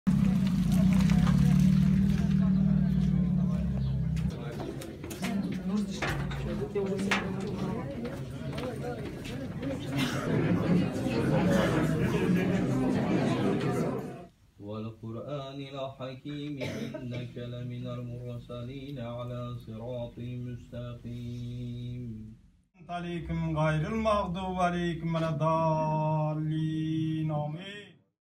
В Бахчисарае молились за возвращение пропавшего Эрвина Ибрагимова
В доме похищенного год назад крымского татарина Эрвина Ибрагимова вечером 24 мая прошел коллективный молебен, или дуа.